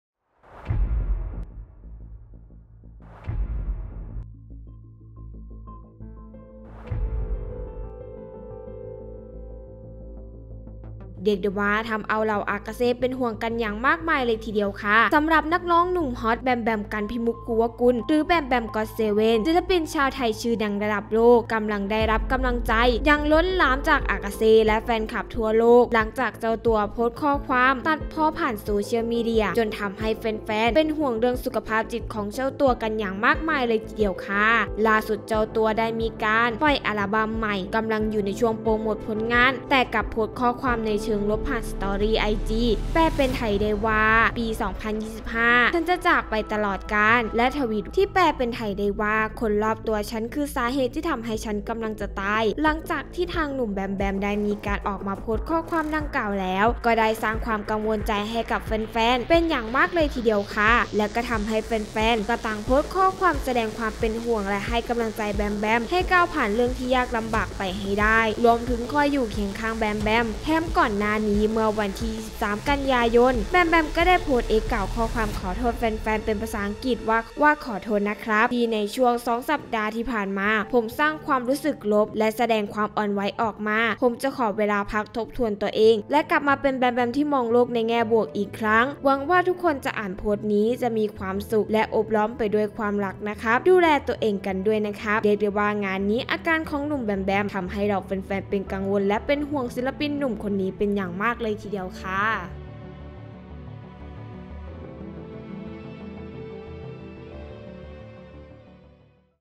อากาเซ่ใจหาย แบมแบม โพสต์ข้อความเศร้า ปี 2025 แฟนคลับแห่เป็นห่วง
เรียกได้ว่าทำเอาเหล่าอากาเซ่เป็นห่วงกันอย่างมากมายเลยที่เดียวค่ะ สำหรับ นักร้องหนุ่มฮอต แบมแบม กันต์พิมุกต์ ภูวกุล หรือ แบมแบม GOT7 ศิลปินชาวไทยชื่อดังระดับโลก กำลังได้รับกำลังใจอย่างล้นหลามจากอากาเซ่ และแฟนคลับทั่วโลก หลังจากเจ้าตัวโพสต์ข้อความตัดพ้อผ่านโซเชียลมีเดีย จนทำให้แฟนๆ เป็นห่วงเรื่องสุขภาพจิตใจของเจ้าตัวกันอย่างมากมายเลยทีเดียวค่ะ